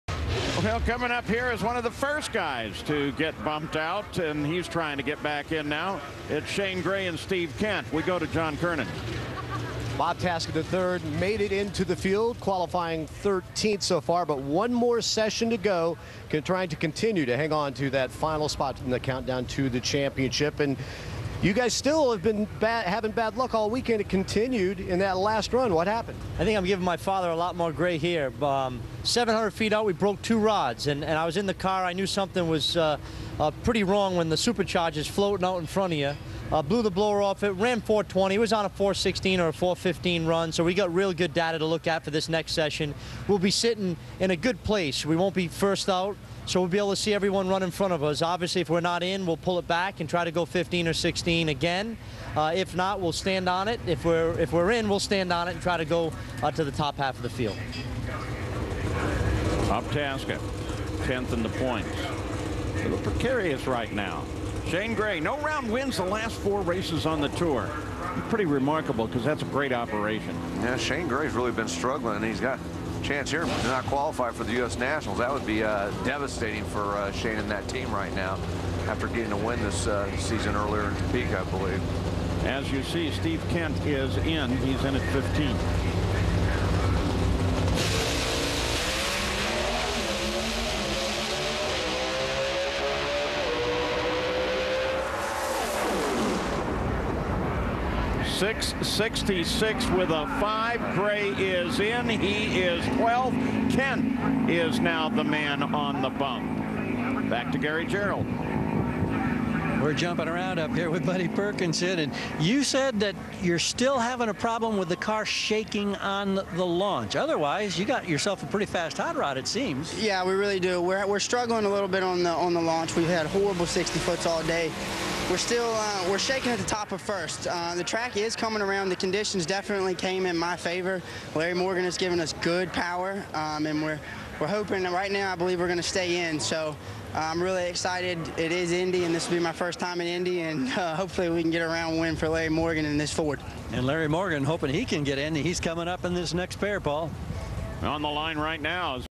Shane Gray Steve Kent Buddy Perkinson Interview Final Session PS MacTools US Nationals 2011.mpg
Shane Gray Steve Kent Buddy Perkinson Interview Final Session PS MacTools US Nationals 2011